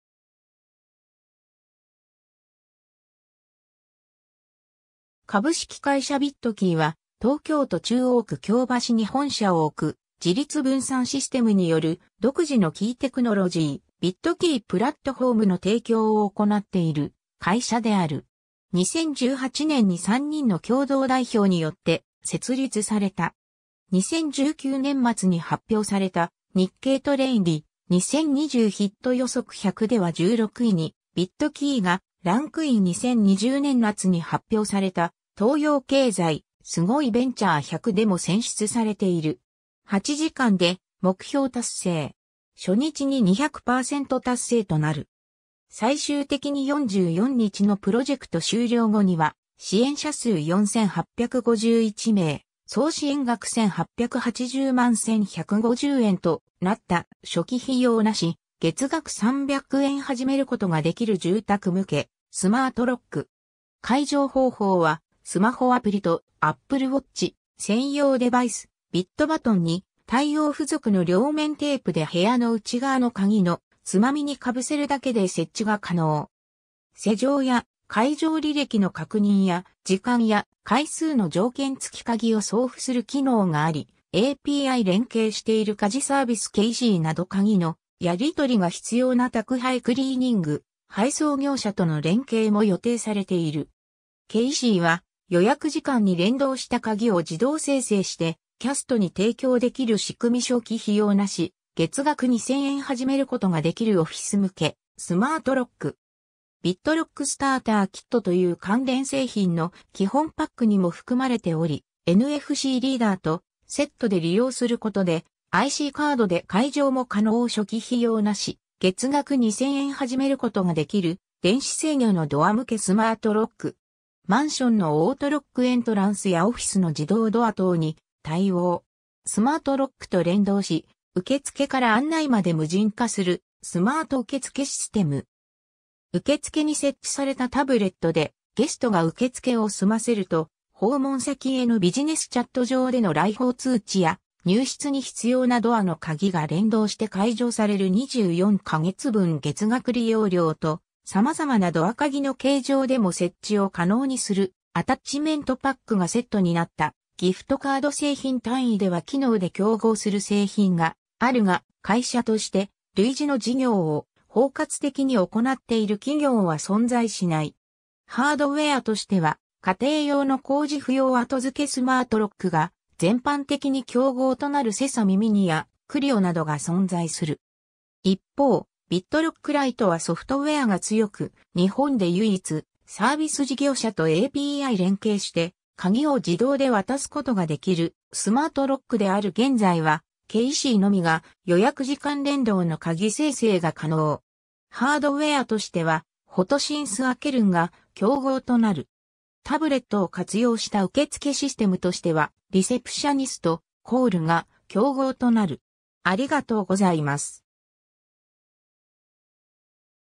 ビットキー
株式会社ビットキーは、東京都中央区京橋に本社を置く、自律分散システムによる独自のキーテクノロジー「bitkey platform」の提供を行なっている会社である。
2018年に3人の共同代表（代表取締役CEO江尻 祐樹、代表取締役COO福澤 匡規、代表取締役CCO寳槻 昌則）によって設立された。
2019年末に発表された日経トレンディ「2020ヒット予測100」では16位に「ビットキー」がランクイン2020年夏に発表された東洋経済「すごいベンチャー100」でも選出されている。
 　　　　　　　　　8時間で目標達成。
初日に200%達成となる。
最終的に 44日のプロジェクト終了後には、 　　　　　　　　　支援者数4,851名、総支援額18,801,150円となった 　 初期費用なし、月額300円〜始めることができる住宅向けスマートロック。
解錠方法はスマホアプリ（iOS＆Android）とApple Watch、専用デバイス「bitbutton」に対応 　　付属の両面テープで部屋の内側のカギのつまみ（サムターン）にかぶせるだけで設置が可能。
施錠や解錠履歴の確認や、時間や回数の条件付きカギ（チケット）を送付する機能があり、API連携している家事サービスCaSyなどカギのやり取りが必要な宅配クリーニング、配送業者との連携も予定されている。
CaSyは予約時間に連動したカギを自動生成してキャストに提供できる仕組み 　 初期費用なし、月額2,000円〜始めることができるオフィス向けスマートロック。
bitlock Starter Kitという関連製品の基本パックにも含まれており、NFCリーダーとセットで利用することで、ICカードで解錠も可能 　 初期費用なし、月額2,000円〜始めることができる電子制御のドア向けスマートロック。
マンションのオートロックエントランスやオフィスの自動ドア等に対応。
 　 スマートロックと連動し、受付から案内まで無人化するスマート受付システム。
受付に設置されたタブレットでゲストが受付を済ませると、訪問先へのビジネスチャット上での来訪通知や入室に必要なドアのカギが連動して解錠される 　24か月分月額利用料と様々なドア・カギの形状でも設置を可能にするアタッチメントパックがセットになったギフトカード 製品単位では機能で競合する製品があるが、会社として類似の事業を包括的に行っている企業は存在しない。
 ハードウェアとしては家庭用の工事不要後付スマートロックが全般的に競合となる　SESAMI miniやQRIOなどが存在する。
 一方、bitlock LITEはソフトウェアが強く、日本で唯一、サービス事業者とAPI連携してカギを自動で渡すことができるスマートロックである 現在はCaSyのみが予約時間連動のカギ生成が可能。
 ハードウェアとしてはフォトシンスAkerunが競合となる。
 タブレットを活用した受付システムとしては、RECEPTIONIST（レセプショニスト）、ACALLが競合となる